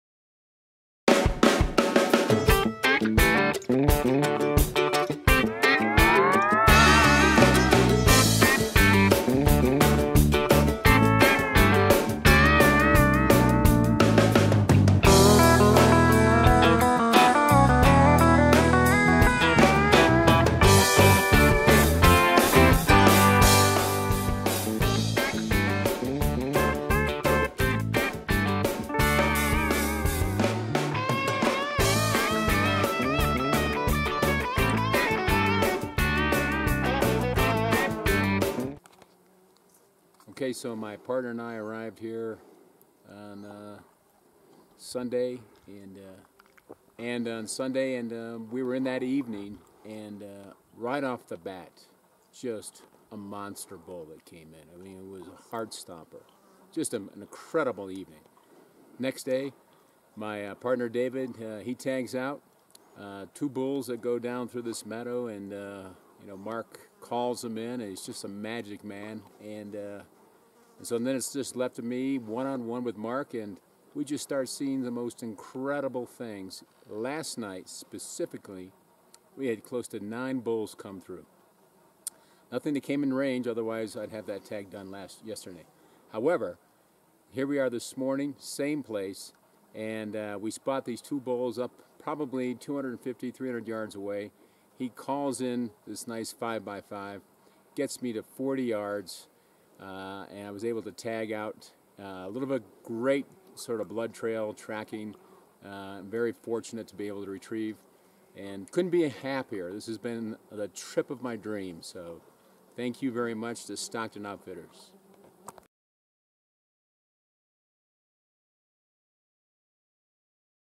Doug Fries (OH) - Montana Archery Elk Hunt
Hunter Doug Fries (OH) harvests his first archery bull elk with guide, Mark Shutey, in South Western Montana. He and his partner David doubled up.